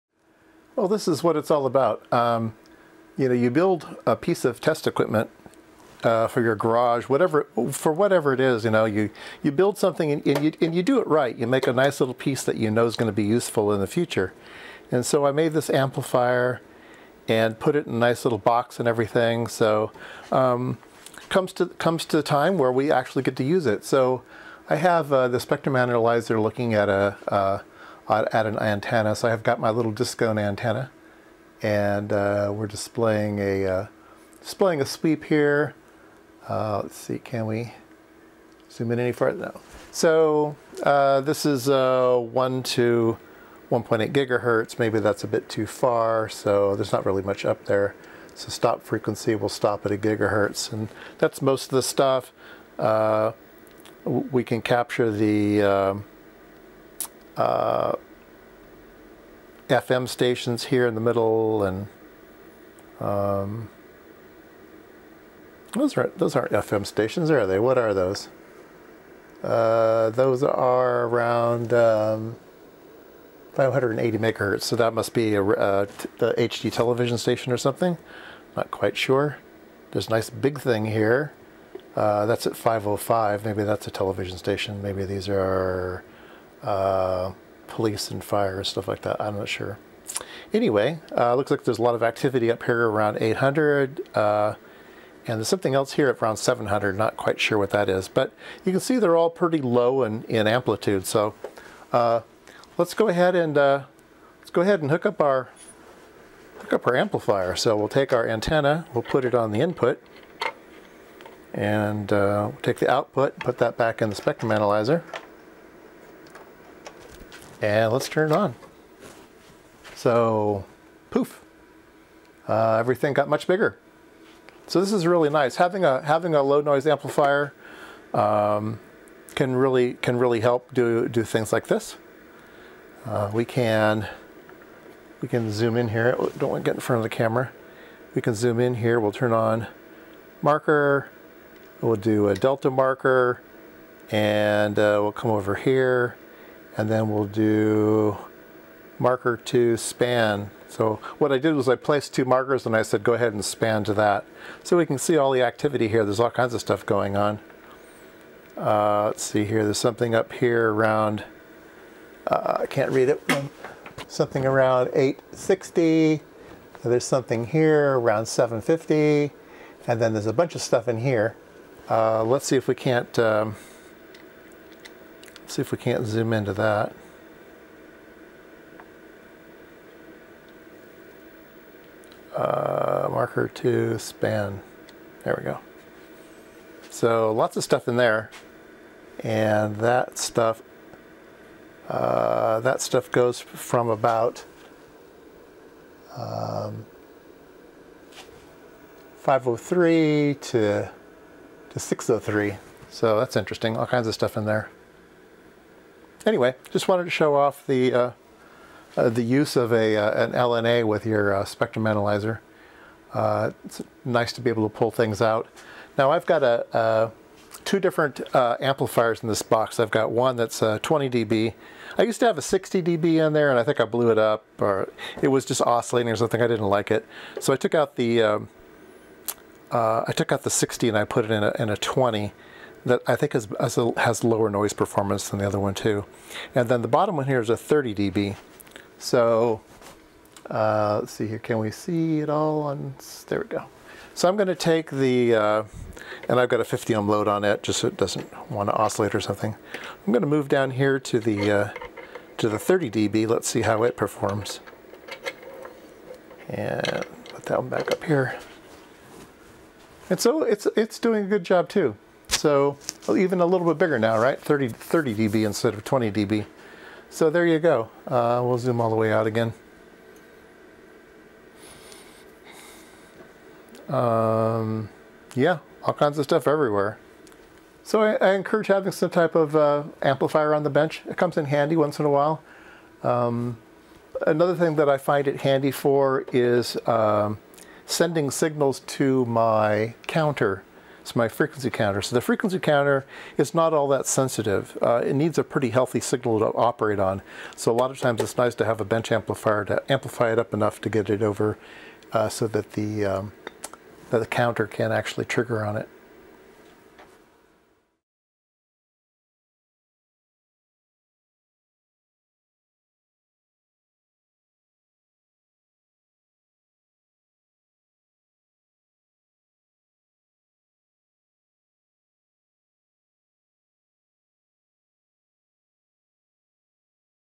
#600 Amplified Signals from Discone Antenna
Episode 600
Using a broadband discone antenna into a low noise amplifier and displayed on a spectrum analyzer
Be a Patron: